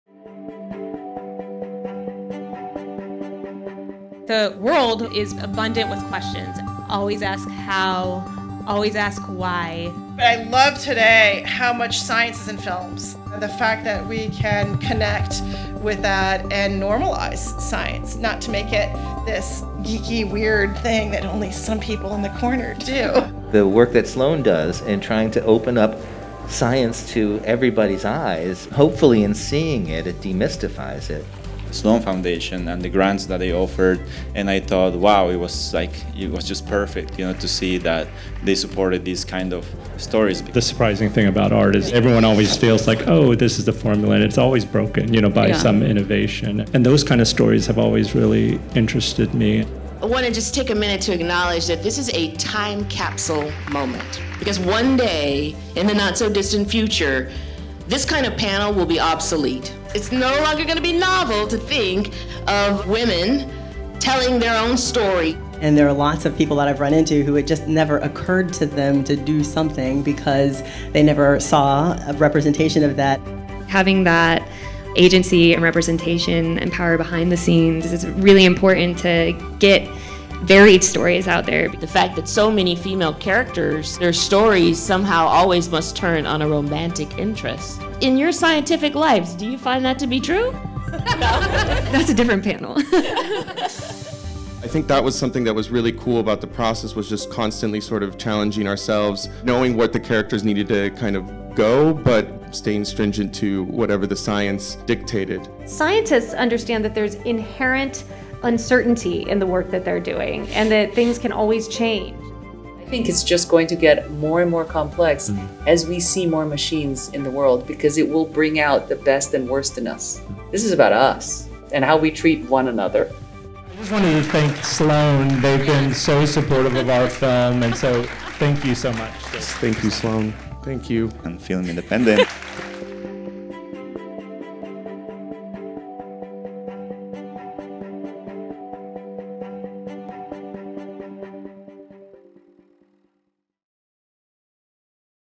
BEST OF THE 2022 SLOAN FiLM SUMMiT | Film Independent
Highlights from the 2022 SLOAN FiLM SUMMiT held April 8-11 in Los Angeles, presented by the Alfred P. Sloan Foundation and Film Independent.